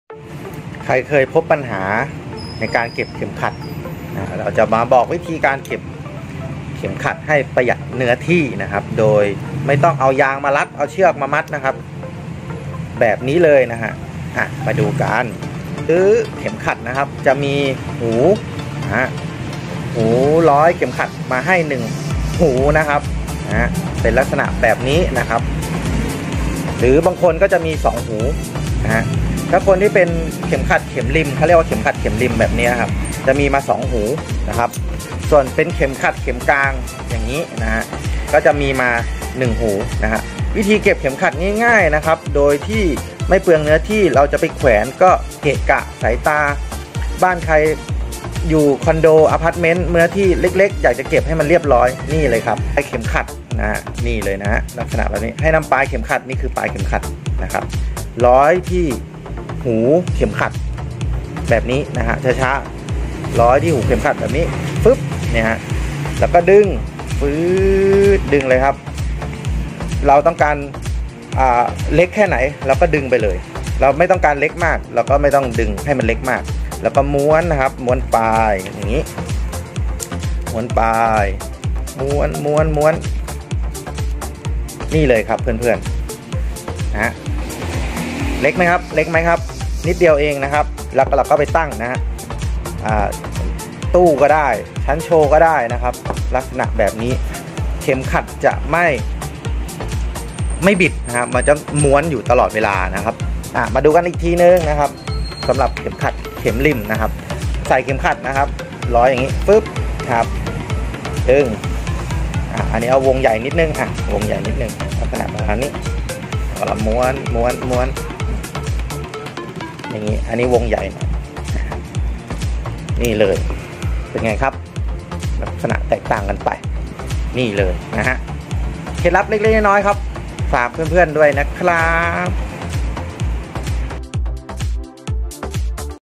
วิธีม้วนเข็มขัด ให้เรียบร้อยแบบง่ายๆ#ร้านสตางค์เครื่องหนังพิดโลก
วิธีเก็บเข็มขัดให้เรียบร้อยรักษาสภาพของหนังใช้ได้ยาวนานไม่เสียง่ายดูคลิปนี้ครับ